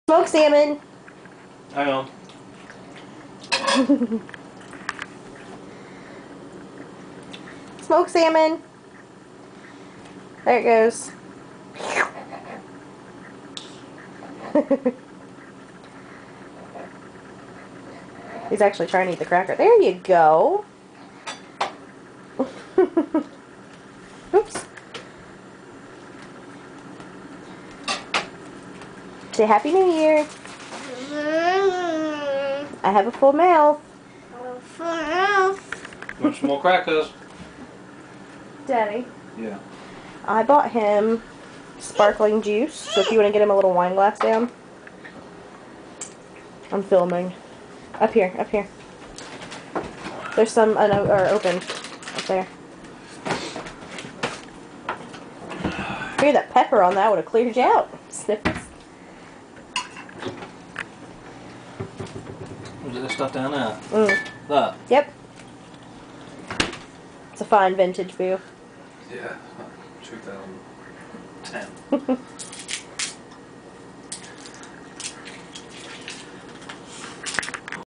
Trying Smoked Salmon for the first time
Daddy likes fish and seafood, so if you're noticing a trend here, that's why.